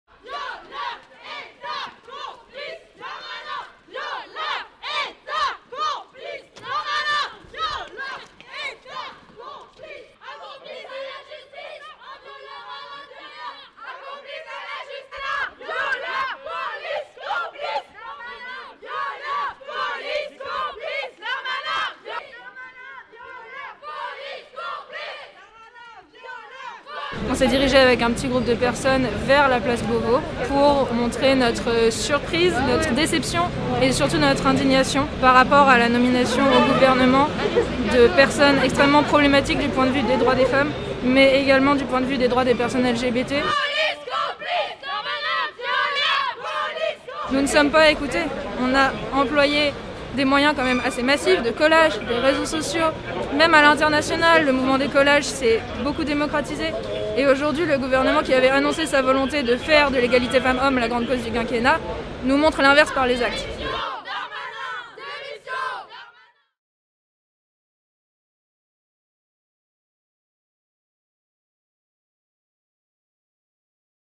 Des féministes manifestent contre les prises de pouvoir de Gérald Darmanin et Eric Dupond-Moretti
Pendant la passation de pouvoir entre Christophe Castaner et Gérald Darmanin, mardi 7 juillet, une vingtaine de militantes féministes s'est rassemblée près du ministère de l'Intérieur, en appelant à la démission du nouveau locataire de la place Beauvau, visé par une plainte pour viol. Les participantes ont également dénoncé la nomination à la Justice d'Eric Dupond-Moretti, qui s'est montré hostile au mouvement MeToo.